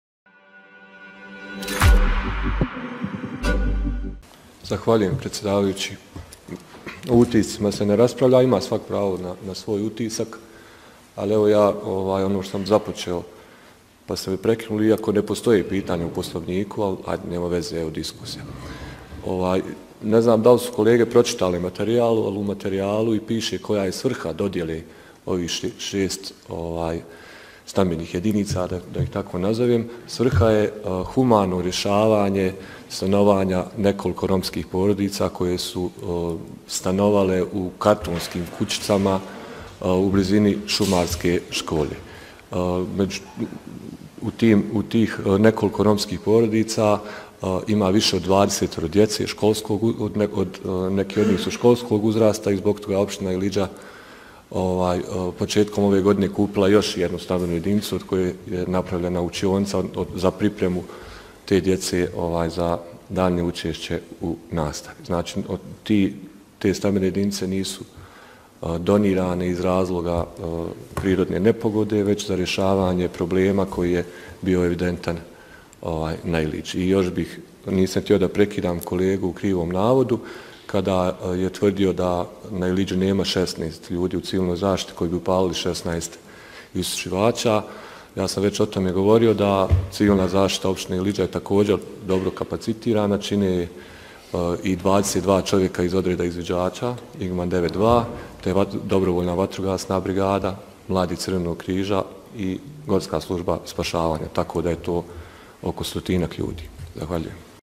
Avdić: Civilna zaštita Općine Ilidža je dobro kapacitirana
Izlaganje Samira Avdića, zastupnika SDP BiH u Skupštini Kantona Sarajevo